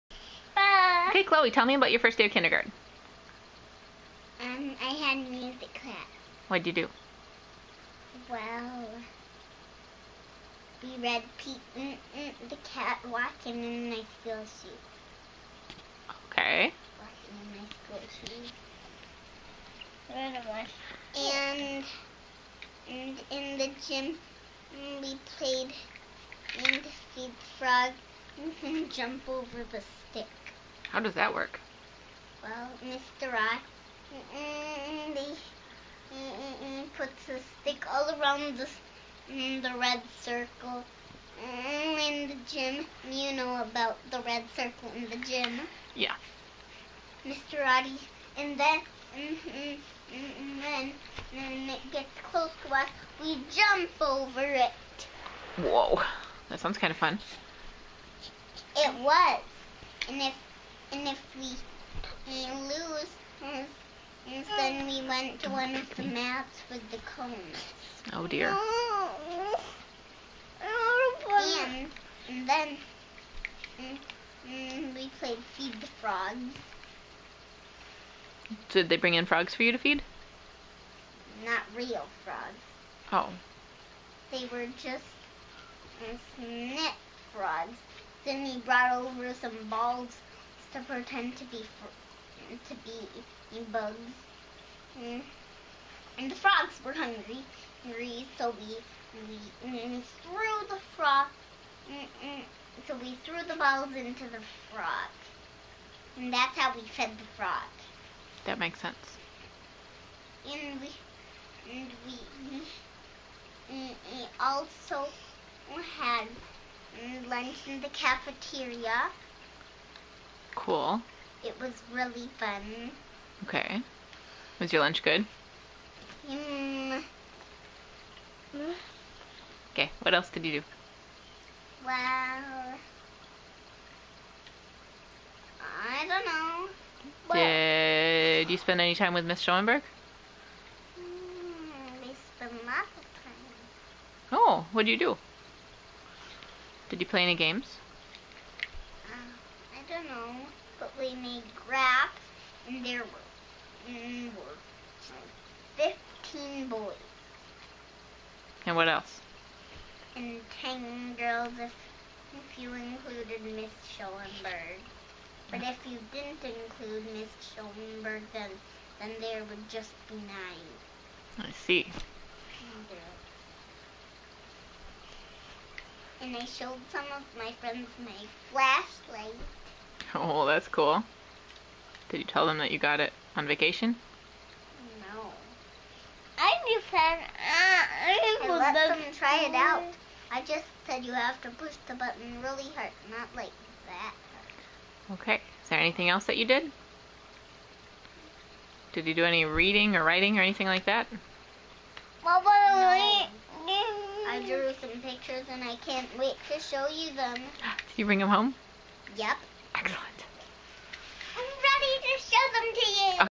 Chloe: First day of K recap
Chloe explaining what happened on her first day of kindergarten